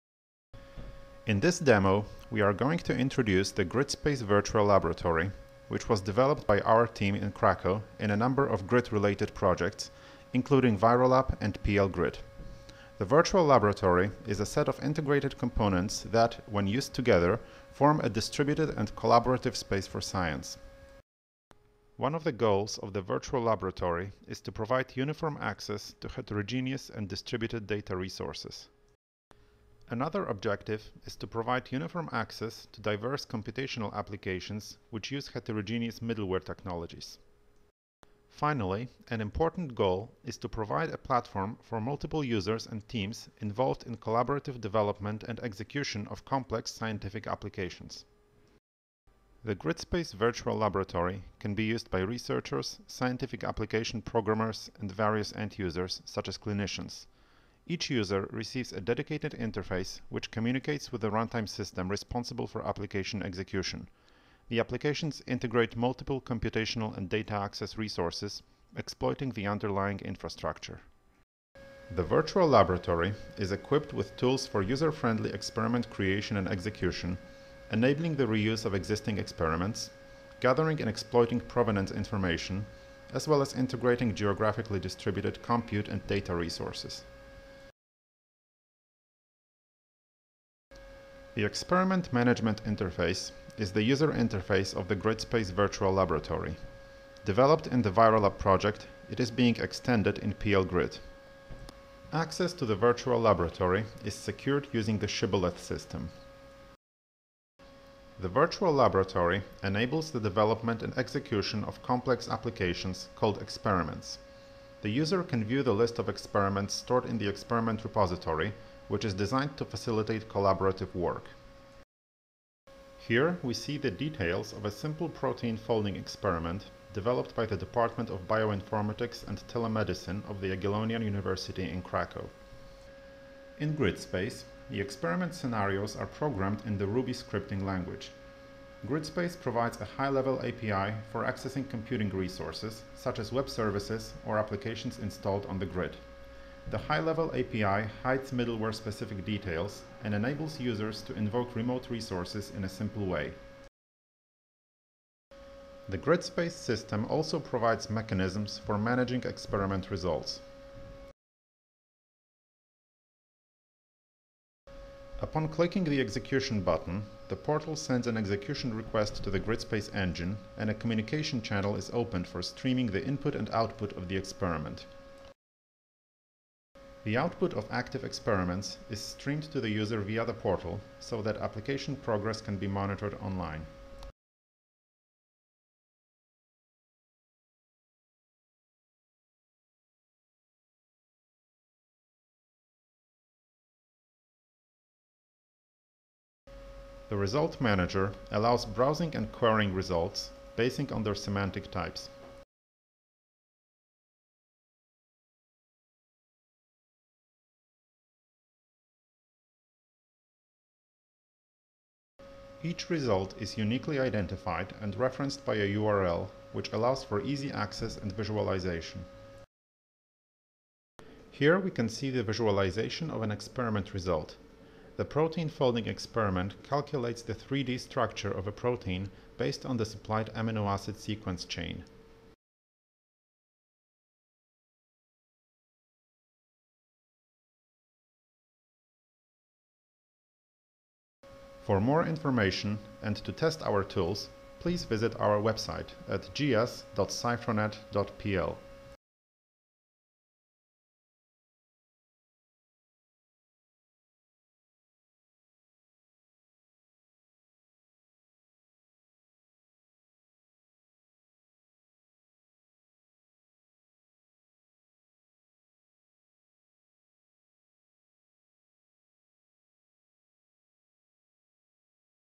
Virtual Laboratory Powered by GridSpace in PL-Grid
We present the details of GridSpace platform which serves
as a basis for the virtual laboratory developed in PL-Grid project.
In GridSpace, the high-level notation for composition of complex
applications is based on the Ruby scripting language.
To provide an easy access to different computing and data resources,
we have introduced a grid object abstraction level hierarchy.
The Virtual Laboratory is equipped with tools for user-friendly
experiment creation and execution, enabling reusing of existing
experiments, gathering and exploiting provenance, as
well as integration of geographically-distributed compute and data
resources.

Virtual Laboratory has been applied to:
- execute important virological experiments in ViroLab,
- protein folding and structure comparison,
- data mining with the Weka library,
- computational chemistry - to develop and run series of
Gaussion application on EGEE.
- as an education tool in computer science classes.